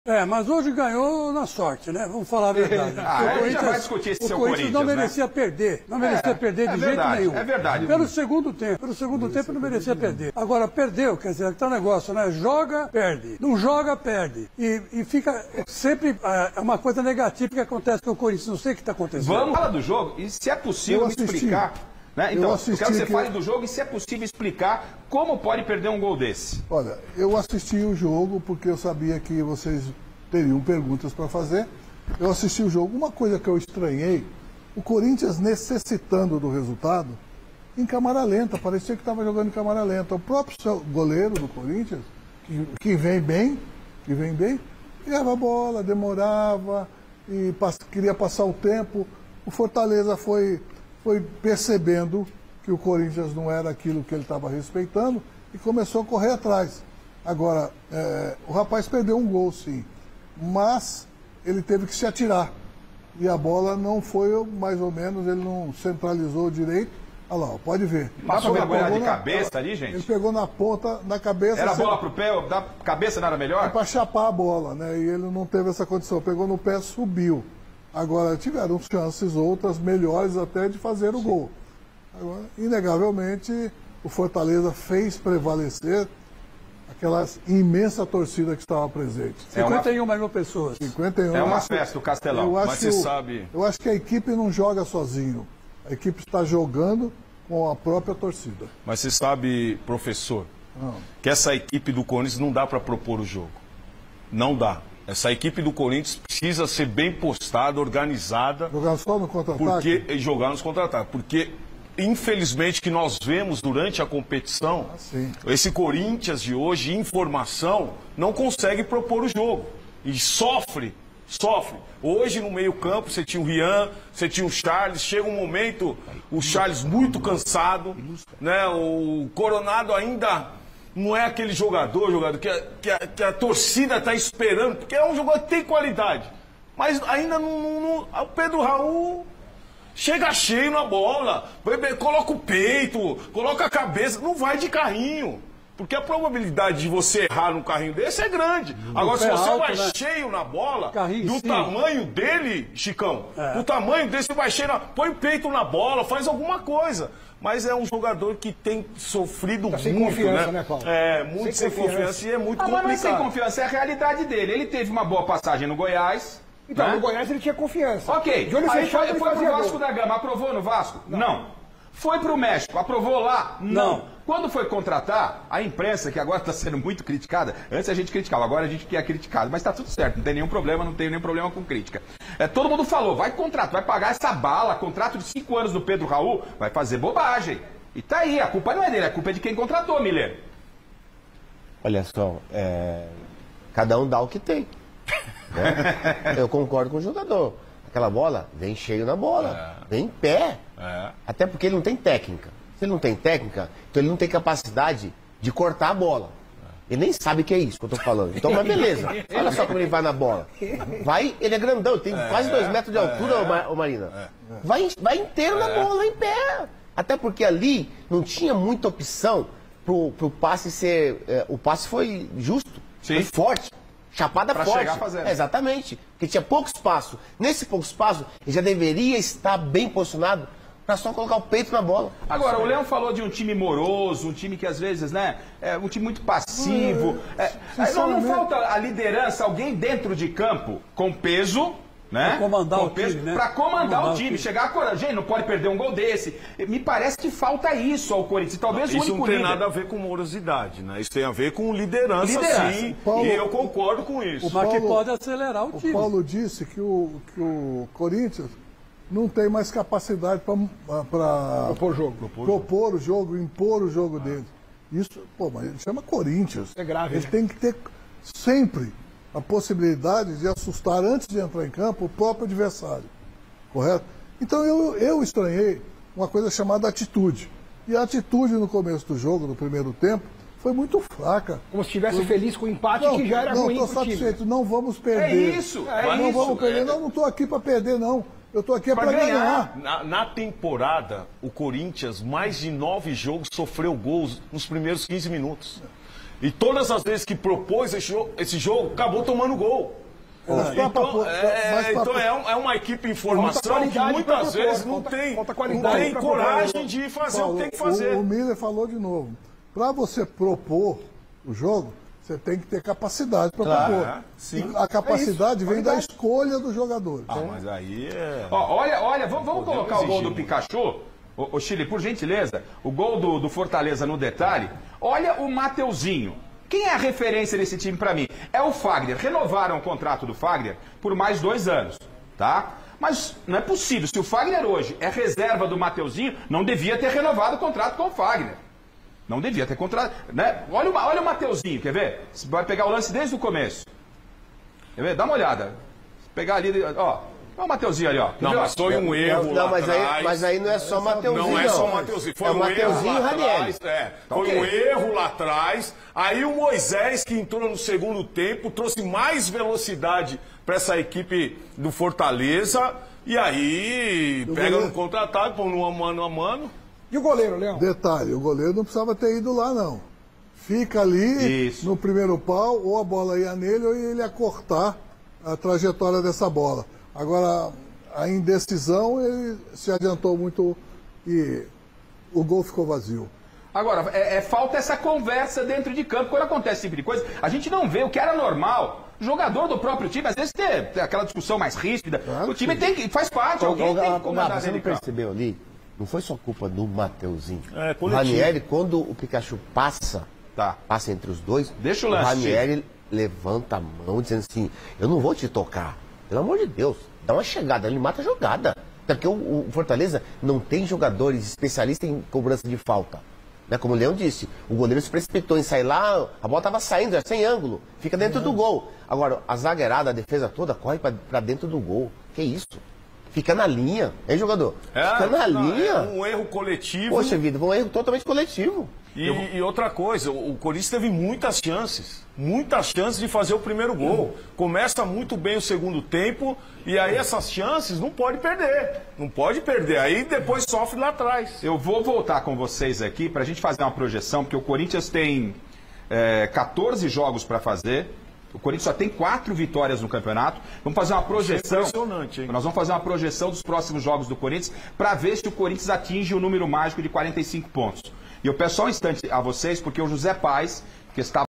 "O Corinthians NÃO MERECEU perder para o Fortaleza", Chico Lang elogia Timão após derrota
O Corinthians perdeu para o Fortaleza no Castelão, mas teve diversas chances para fazer gols e sair da zona do rebaixamento do Brasileirao. Qual foi sua opinião?